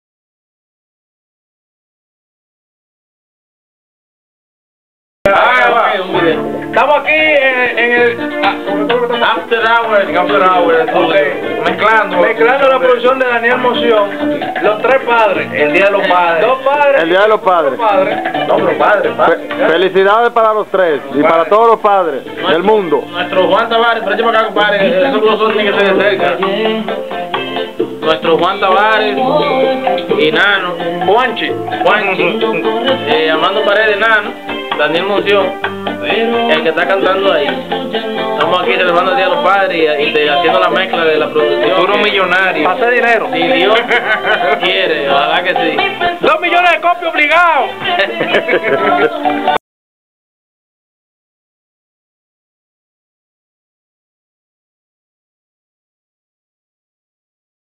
Daniel Moncion con imperio nano en el estudio mezclando
el productor imperio nano en el estudio grabando el disco de daniel moncion